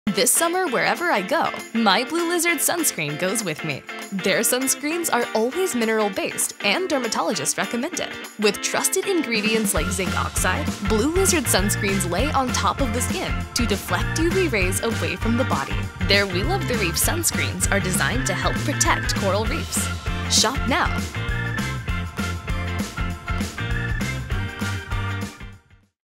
Blue Lizard Sunscreen with consciously selected ingredients :30 - Shop Now
Wherever I go, my Blue Lizard Sunscreen goes with me.

Their sunscreens are always mineral-based and dermatologist recommended.

With trusted ingredients like Zinc Oxide, Blue Lizard sunscreens lay on top of the skin to deflect UV rays away from the body.

Their We Love the Reef sunscreens do not contain Oxybenzone and Octinoxate to help protect coral reefs.